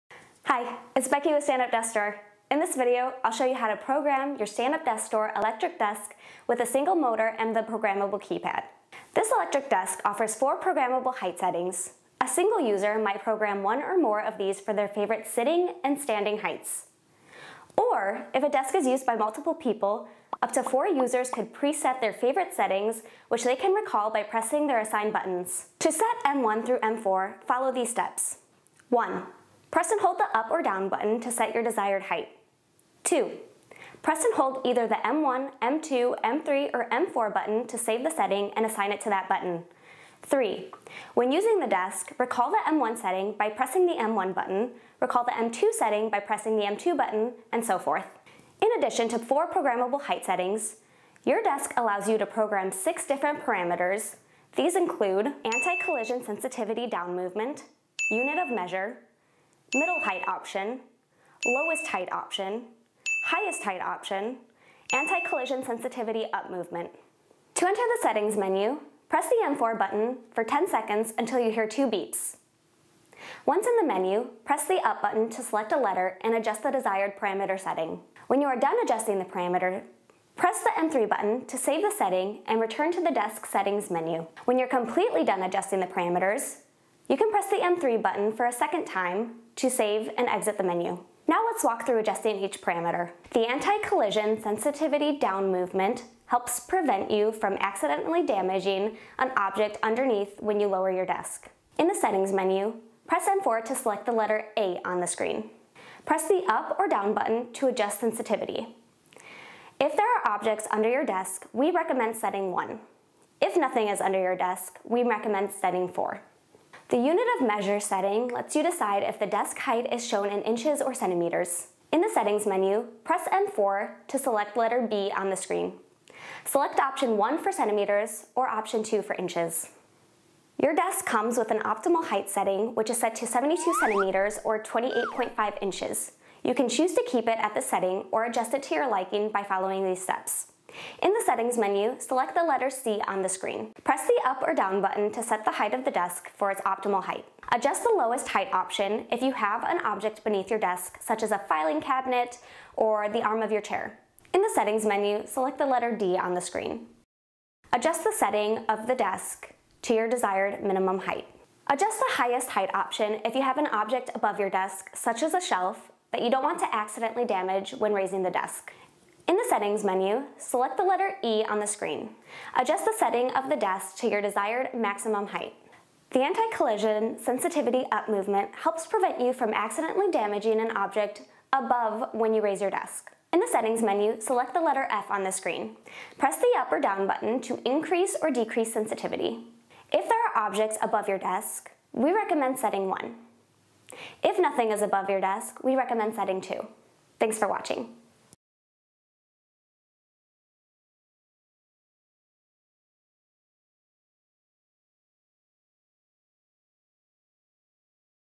How to Program a Stand Up Desk Store Electric Standing Desk (Single Motor 48" and 60" Models)
This video shows you how to program Stand Up Desk Store electric desks with a single motor and a programmable keypad. These include our 48” and 60” models with 3-stage telescoping legs.

Topics covered include:
-- Programming the anti-collision safety function
-- Selecting your unit of measure
-- Programming the default middle/lowest/highest height settings
-- Saving your favorite standing and sitting height settings for instant recall

This video applies to your desk model if:
-- Your desk's part number starts with either SUDE60FT, SUDE60, SUDE48FT, or SUDE48
-- The programmable keypad has a slanted front
-- The keypad buttons are (in this order) Up/Down/M1/M2/M3/M4